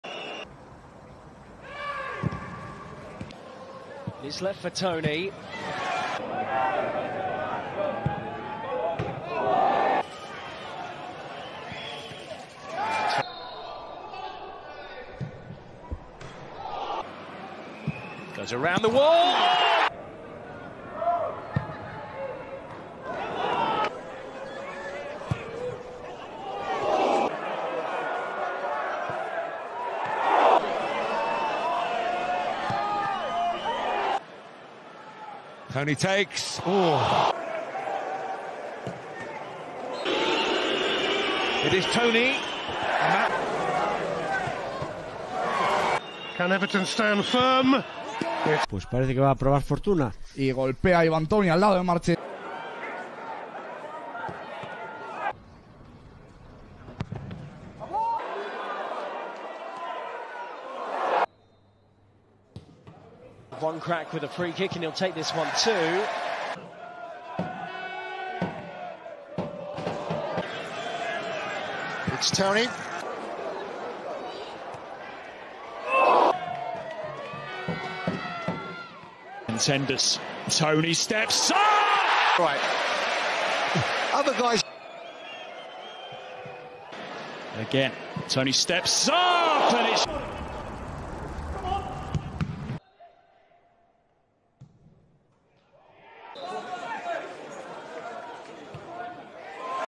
I found all of Ivan Toney's free kicks...
🤩 Toney achieved an incredible goal yesterday when he returned to play.